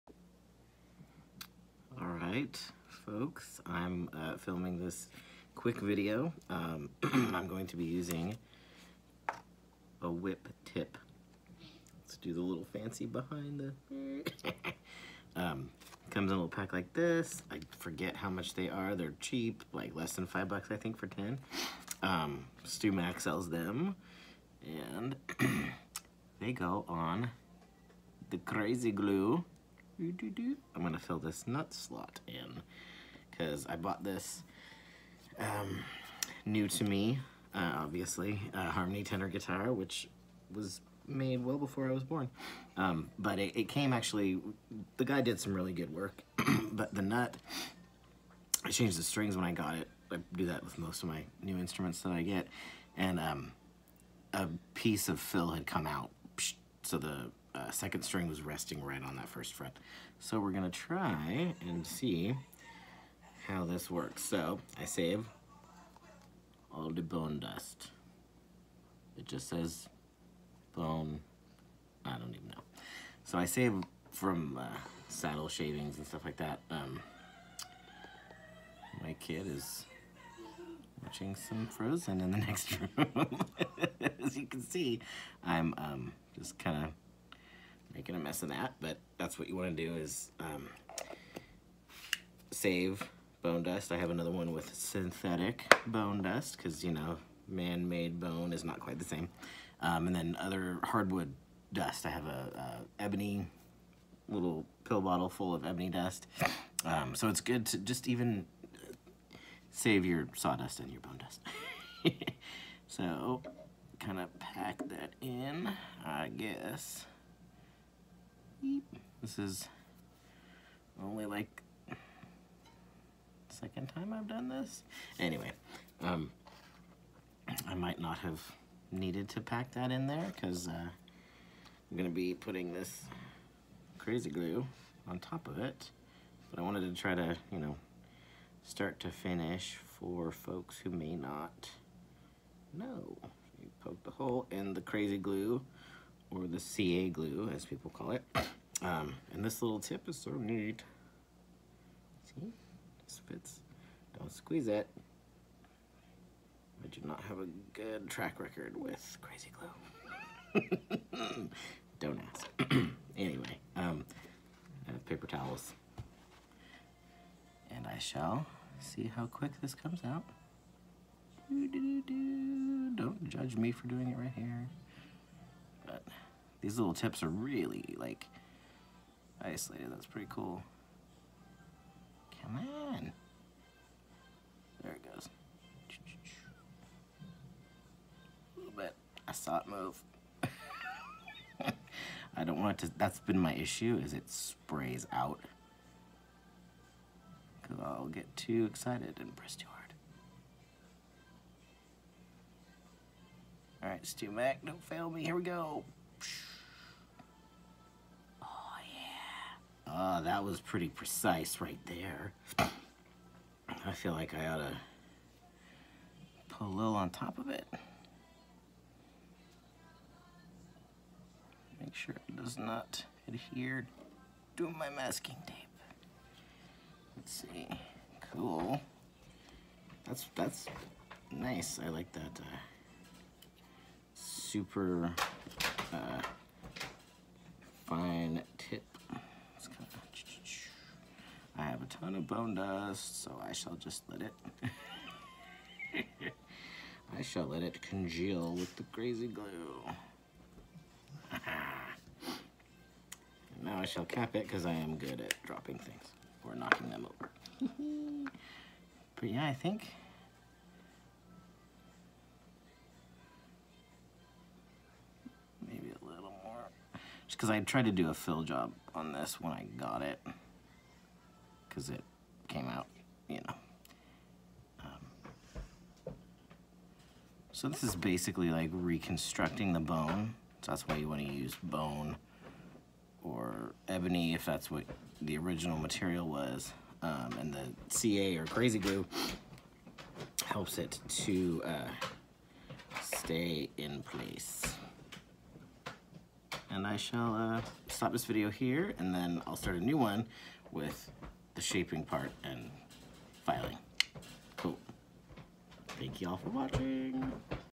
StewMac Whip Tip Demo
Here is a quick demo of my first time using the whip tips from StewMac...definitely impressed since I’m rather accident prone when it comes to CA glue.